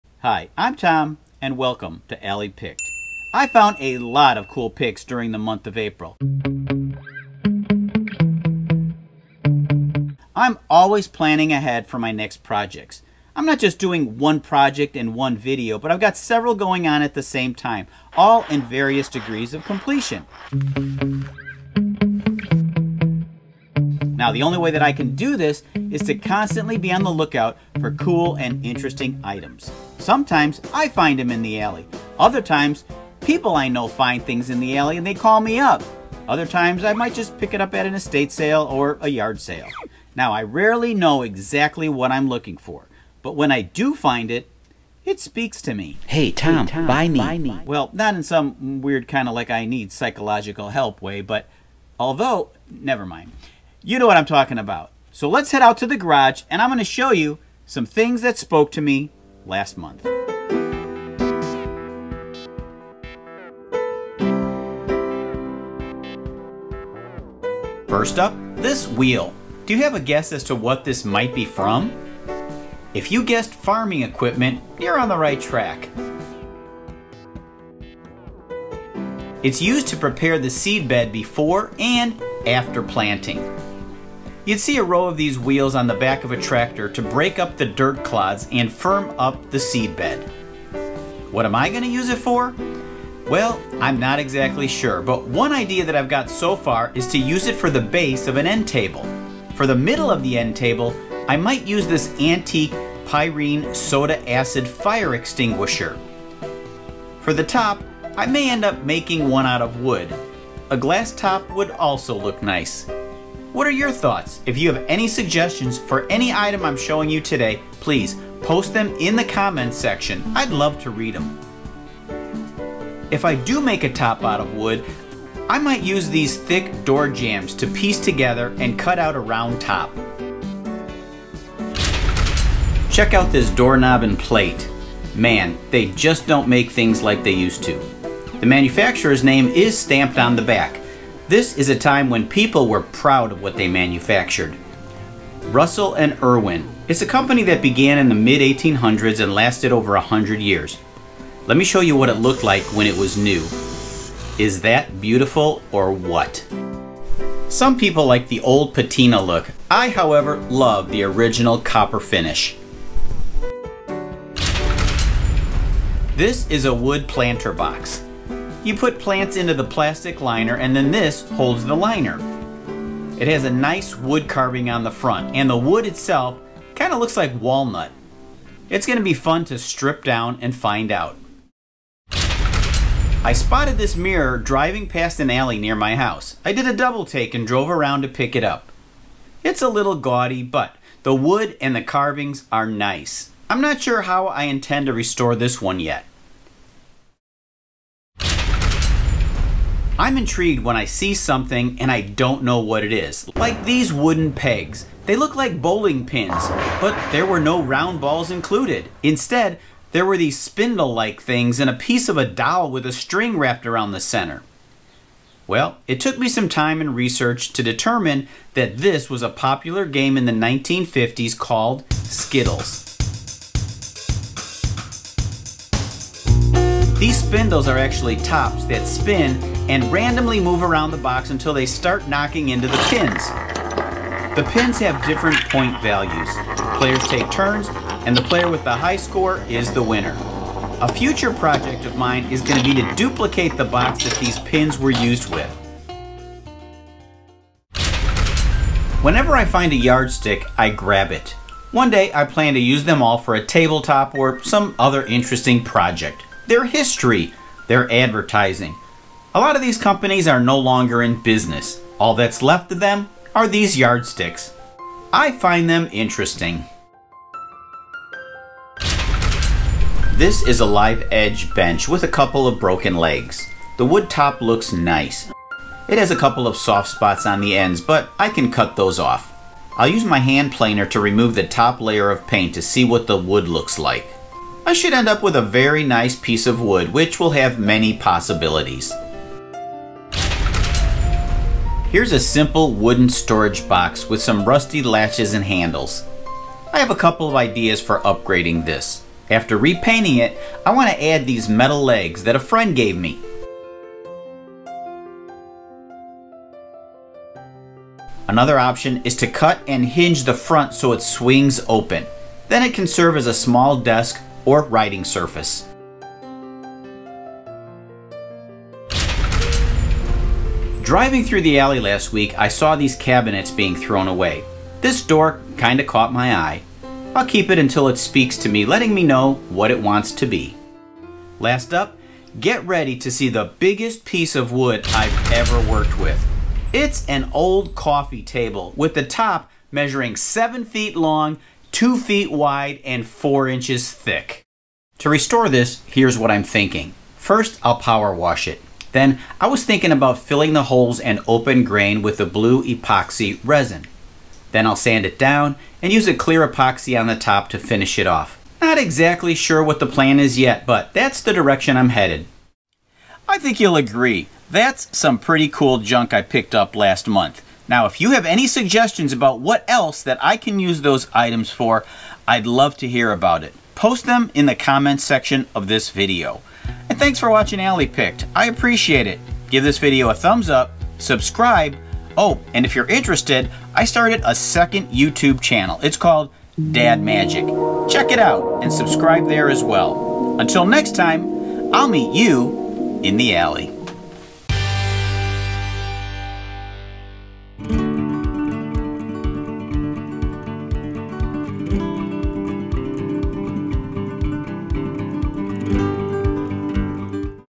Antiques & Picks - 12 Cool things I found!
Last month, I found lots of cool items to upcycle, refinish, and build including a live edge coffee table, antique fire extinguisher, a cultipacker wheel, skittle game, and much more. Check out this video for a a look back at some amazing vintage items.